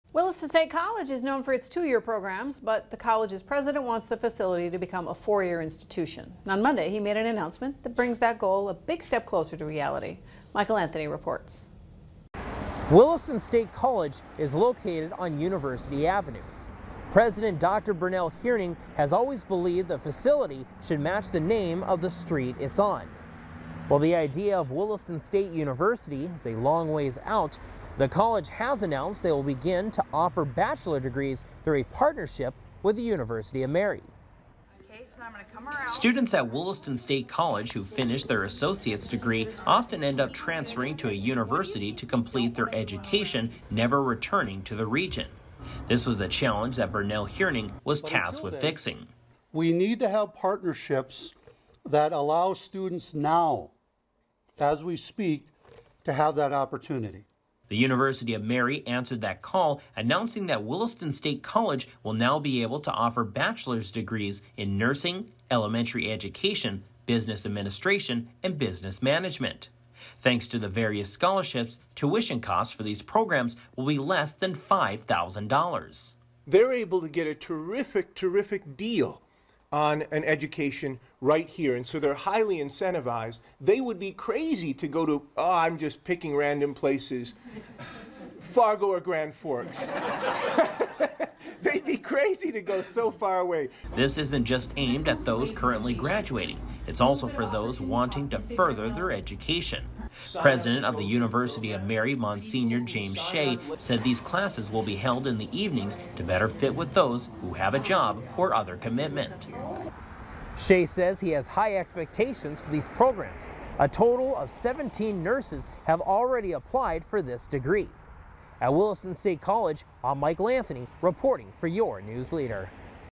Williston State College to offer bachelor’s degrees on-campus thanks to partnership with Universi...
Williston State College to offer bachelor’s degrees on-campus thanks to partnership with University of Mary